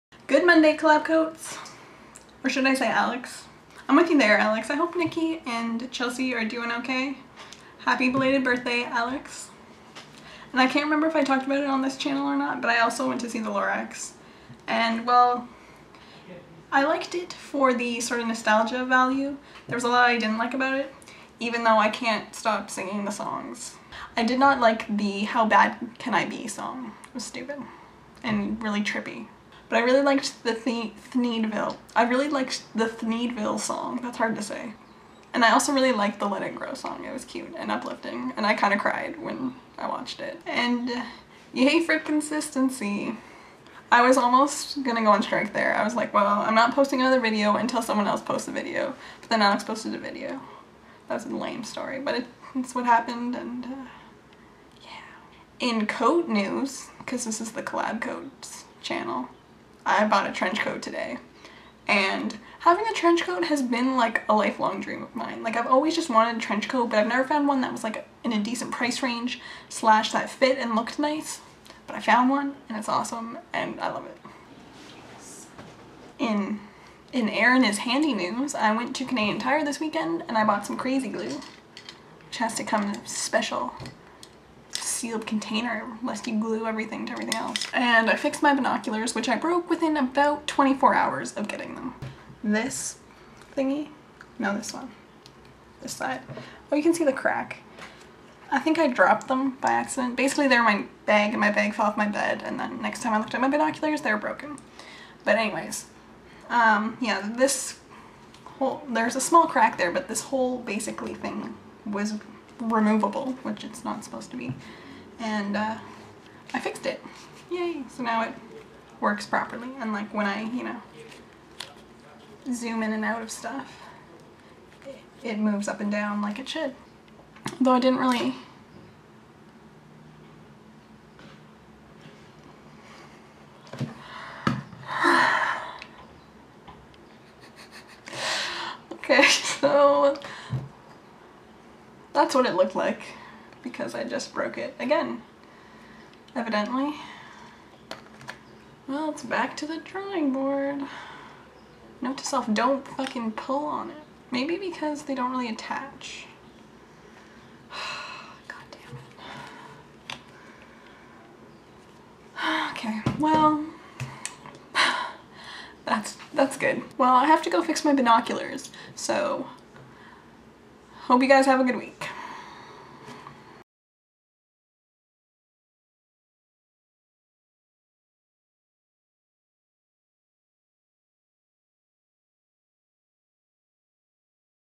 I broke my binoculars...
In which Erin fails comically at being handy. The segment titled "In Erin is handy news" is hereby cancelled indefinitely. There's also some exciting coat news!